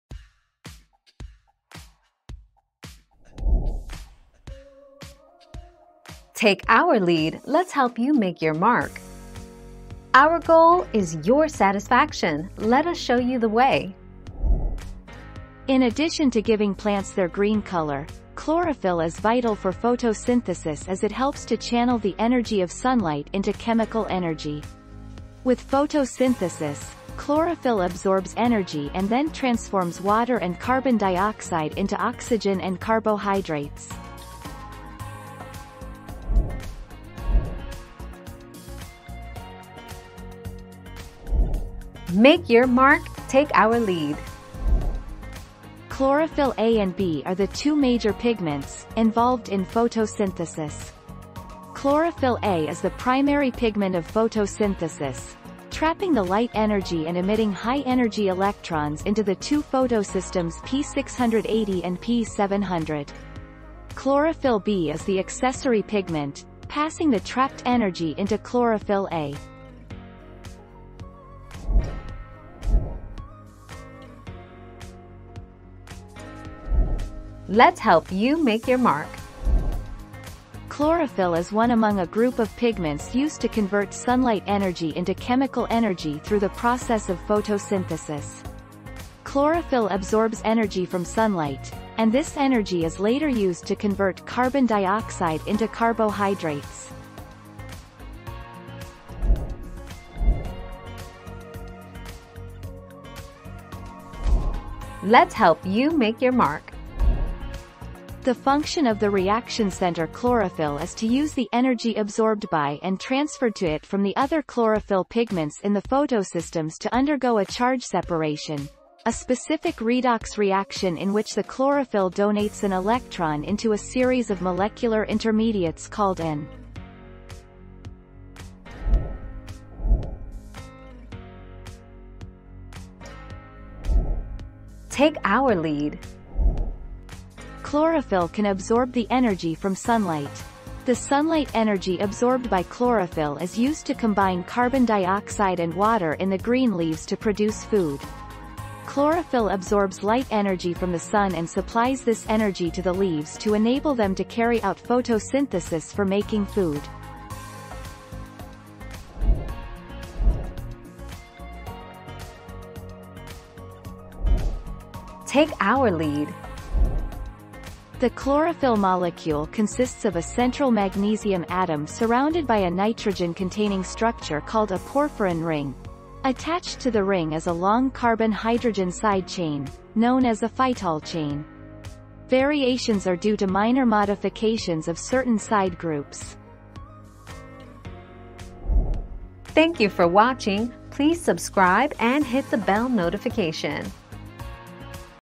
What are the three functions of chlorophyll?
00:00 - What are the three functions of chlorophyll?
00:38 - What is the main function of chlorophyll a and b?
01:13 - What is the function of chlorophyll for Class 8?
01:40 - What is the function of the reaction center chlorophyll?
02:10 - What is the function of chlorophyll class 7?
02:40 - What are the characteristics of chlorophyll?

Laura S. Harris (2021, May 8.) What are the three functions of chlorophyll?
    AskAbout.video/articles/What-are-the-three-functions-of-chlorophyll-242554